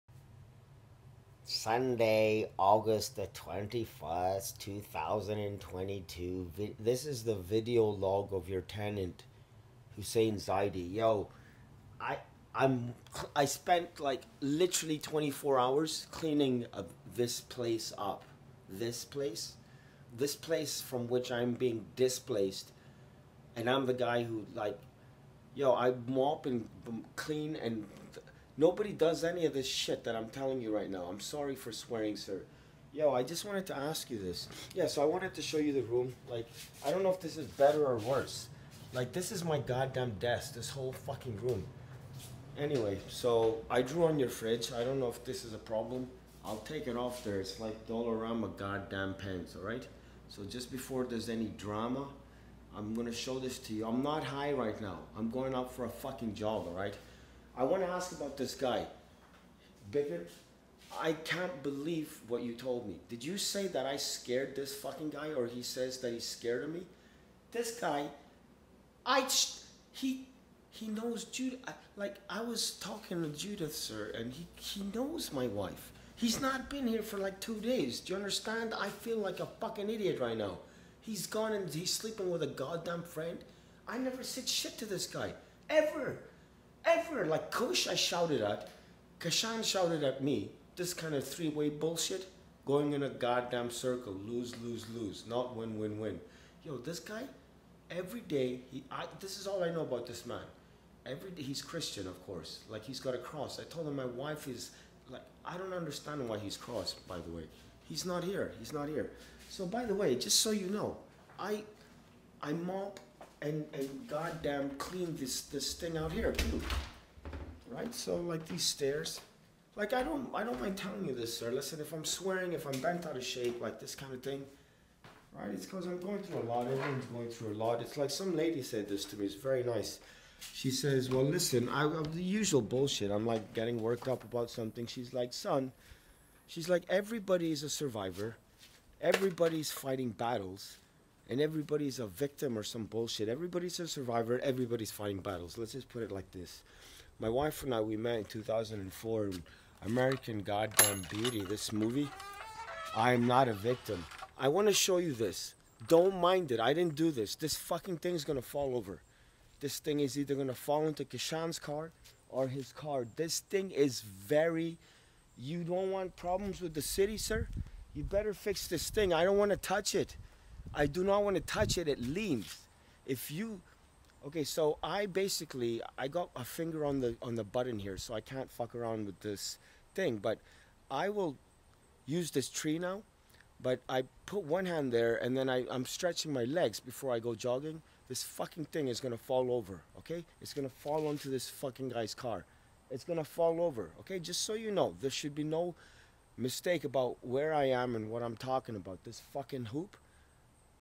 VIDEO LOG l SUN21AUG22 | A Note to My Landlord (Markham & Steeles, Toronto)
this man is into something called “mEcHaTrOniX”

this “thing” is a cross between ELECTRONiX + MECHANiX

it is like Will Ferrell: “It’s a REAL science, with books and EVERYTHING”

he (my landlord) needs to get online and CREATE content about it

(he watches these God damn things, what’re you gonna do, gotta do sOmEtHiNg)

OFF TO MiLLiKEN PARK FOR MY DAiLY JOG l COMMENTS APPRECiATED